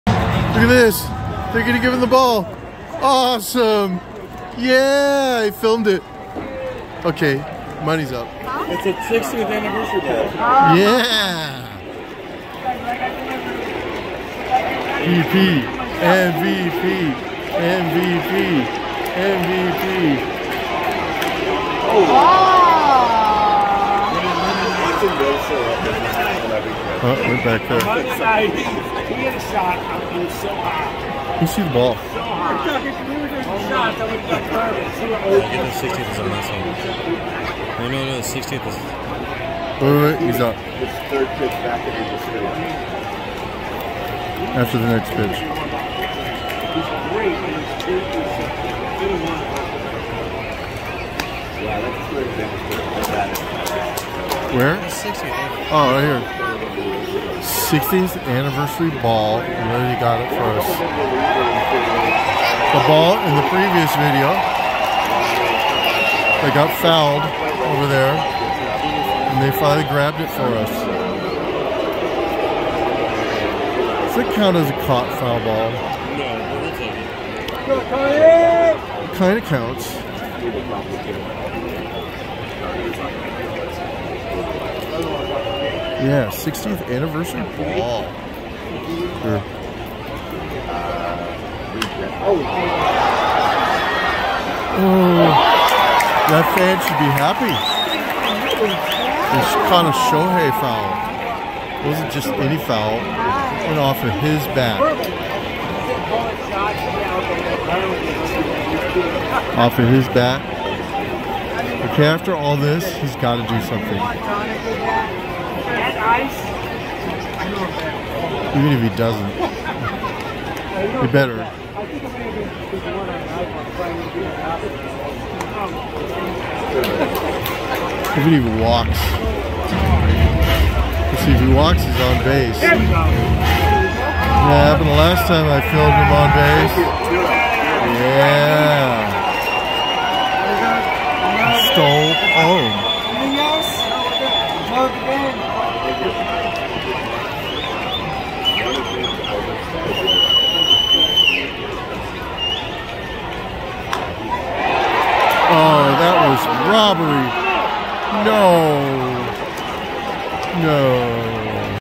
Shohei Ohtani laser caught foul ball
That ball from the last video an usher got it for us!  A 60th anniversary ball! Oh and Shohei Ohtani hit a laser, but got robbed of a hit again.

If you're going to shop on Amazon anyway, why not click on this link and help me out?

このコンテンツが気に入ったら、このビデオを気に入ってこのチャンネルを購読してください!